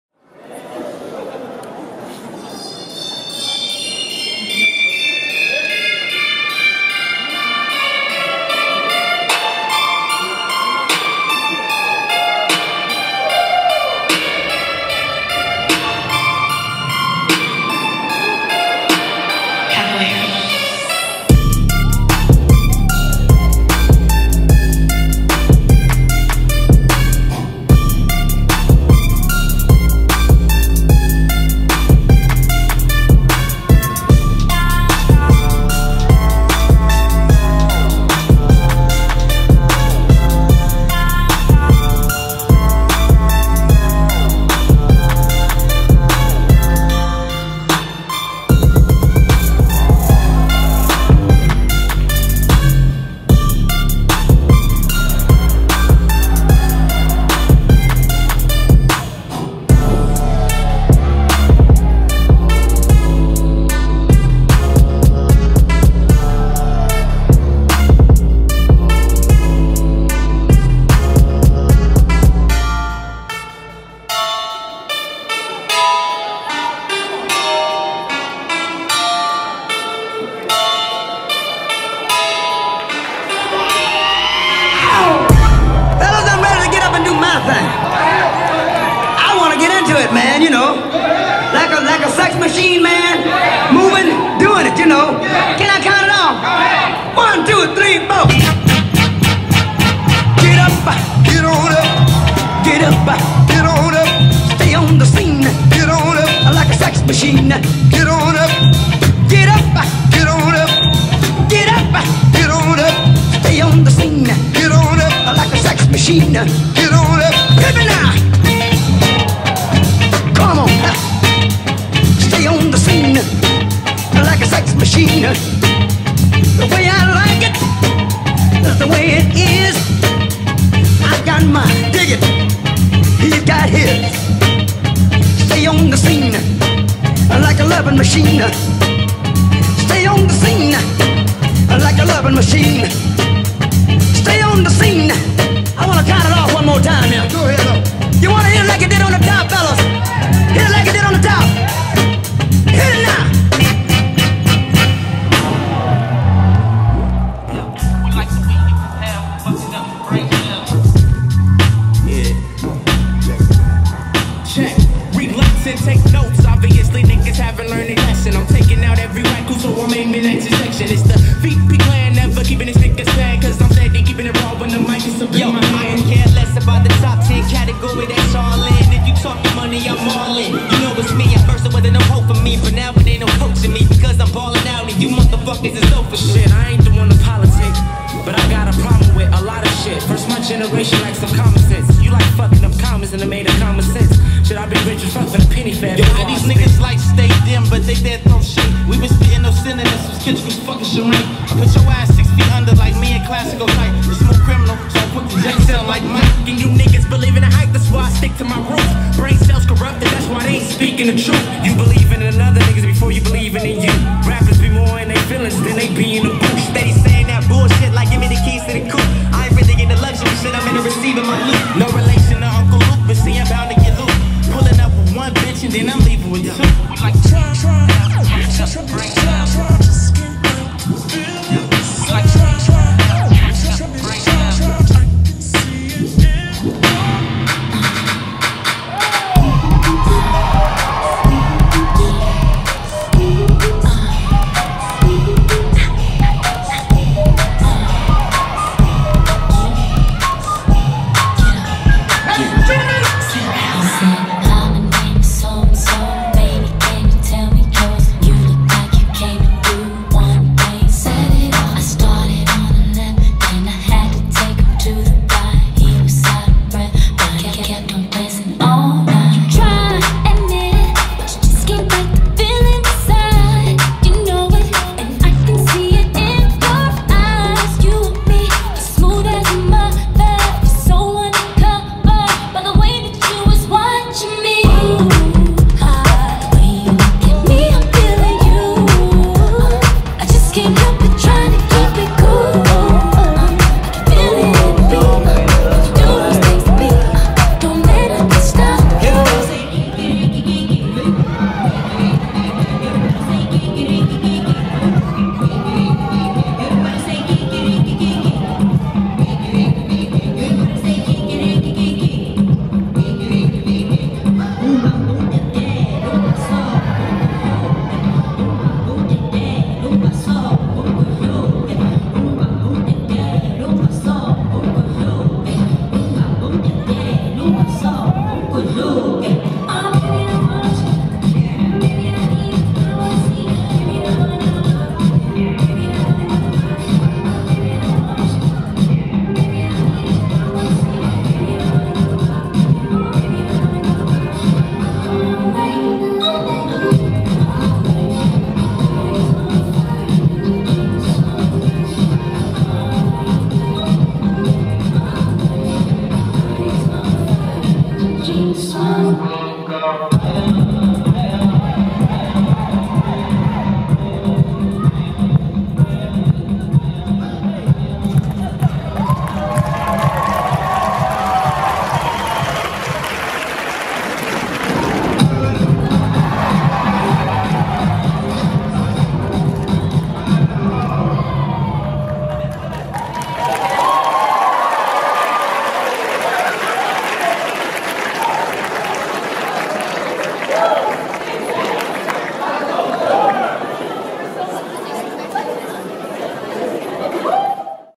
Culture Shock SD | Fusion XVIII 2018 [@VIBRVNCY 4K]
Culture Shock SD | Exhibition
Filmed & Edited by Eryn Aquino @erynrae & Gerald Nonato @geraldnonadoez
Fusion XVIII 2018 presented by MASA
Golden Hall, San Diego 4.7.18